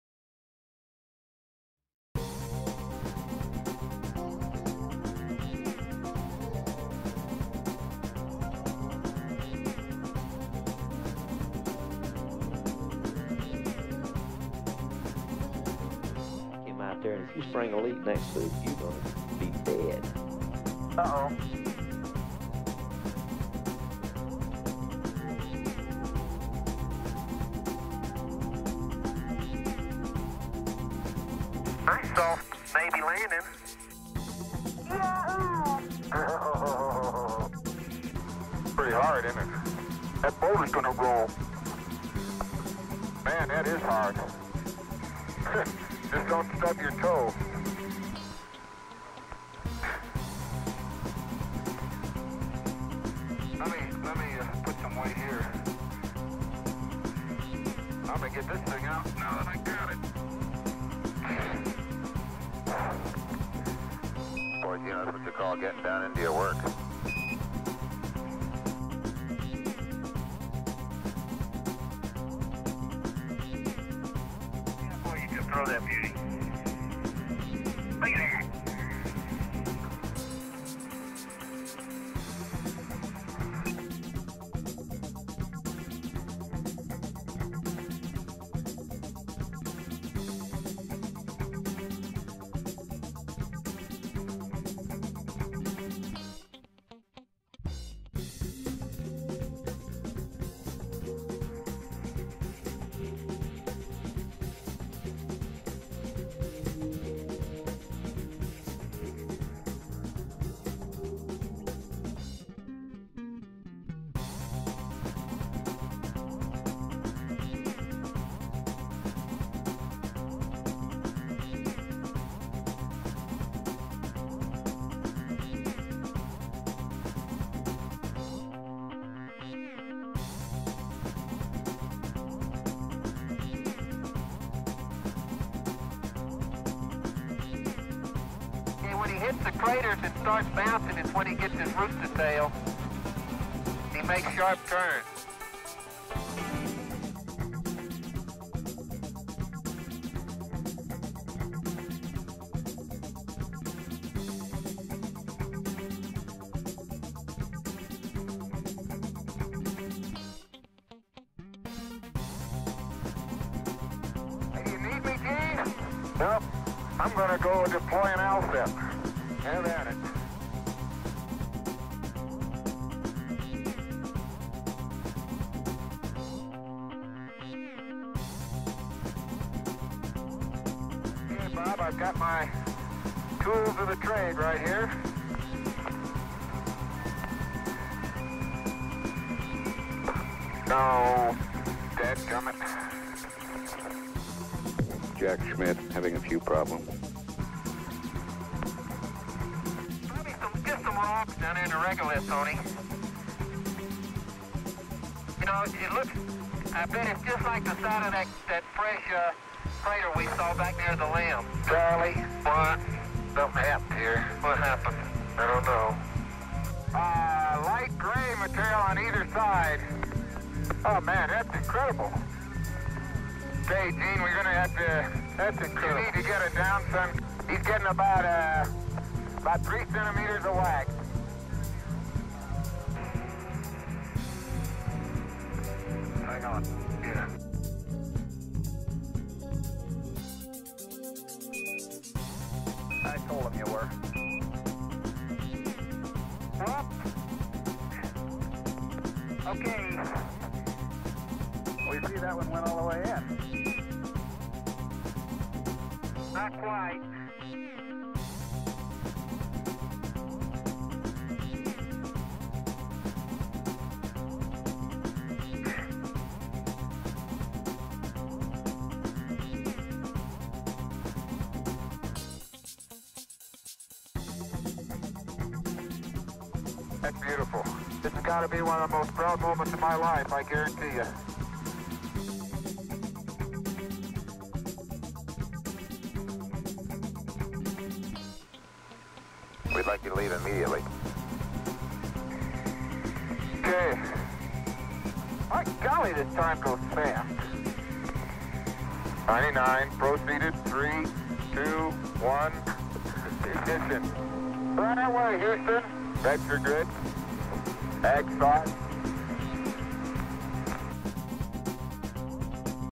Astronauts on the Moon, Throwing Stuff & Falling Down, Lunar Rover, Moon Buggy
Raw video from the NASA archive of Astronauts on the Moon. A funny complication of objects being thrown,  slips and falls, Lunar Rover rooster tails, and having a good time on the Moon.